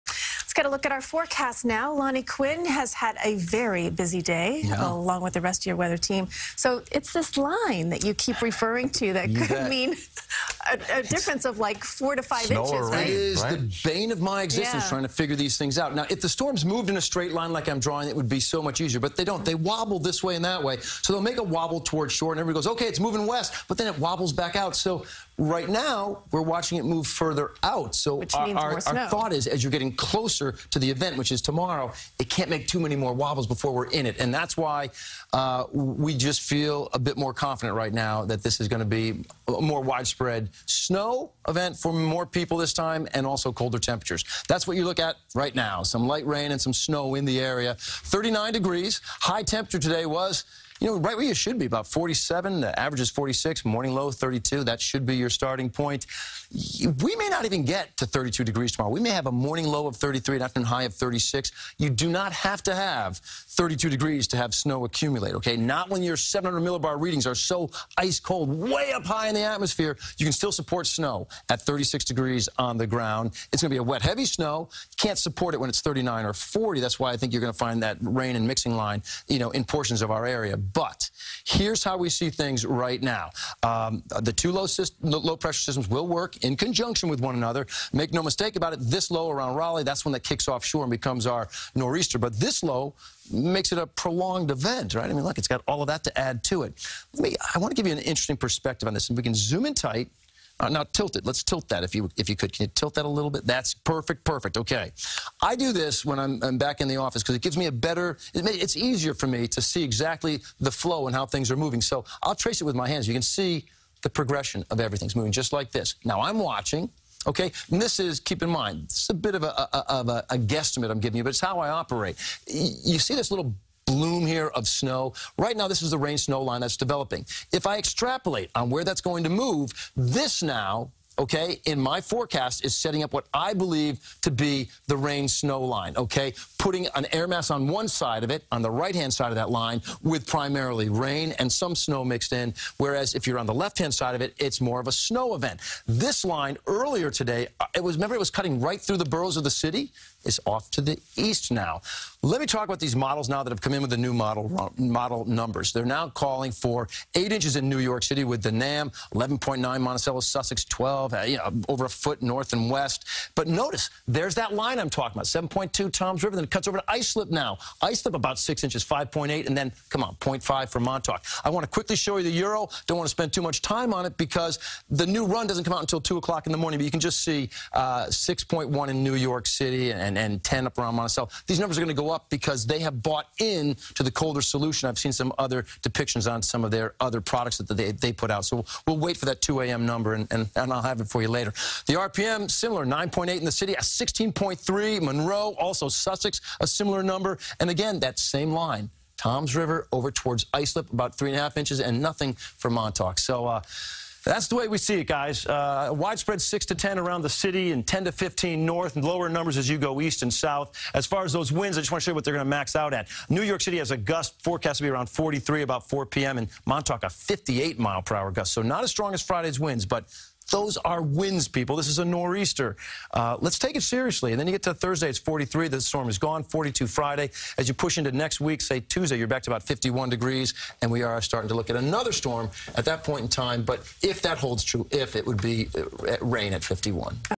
Possible 2nd Nor'easter Bearing Down On The Tri-State
CBS2's Lonnie Quinn has the latest on what you can expect.